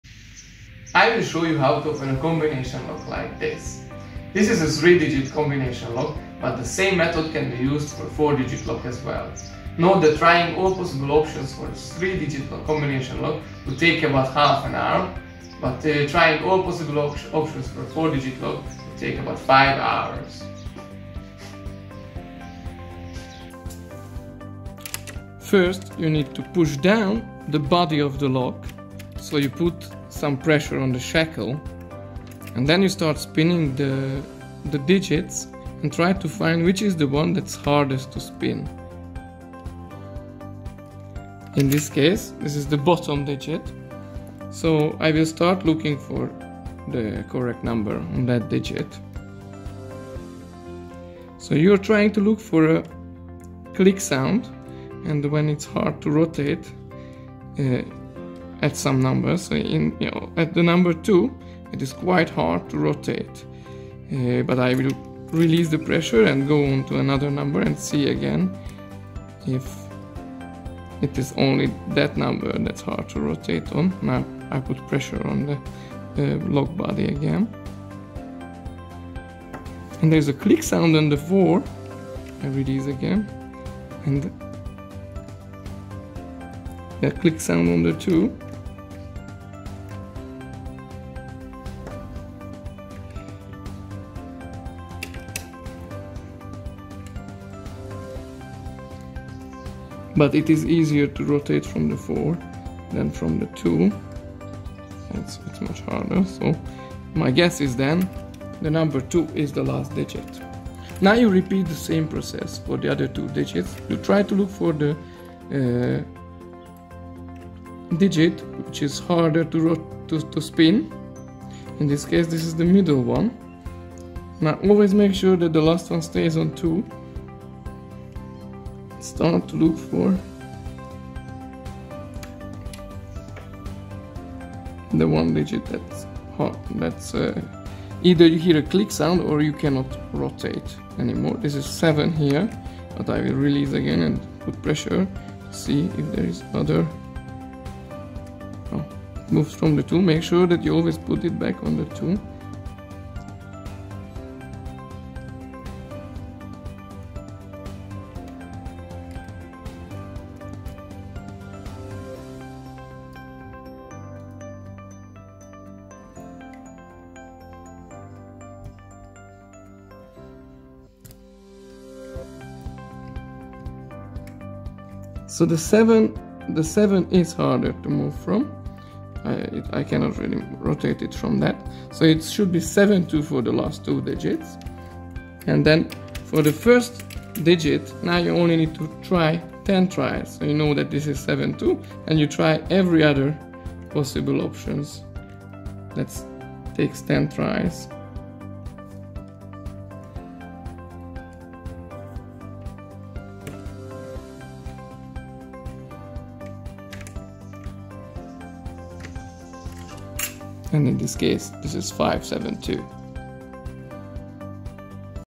How to crack a combination lock with no tools
In this video I explain how one can unlock a combination lock without the code. It may be useful if you forget your combination code. At the same time the video also raises awareness how easy it is to open a padlock without tools. Never trust that a padlock is really protecting your belongings. Lock-picking a key padlock is also not complicated.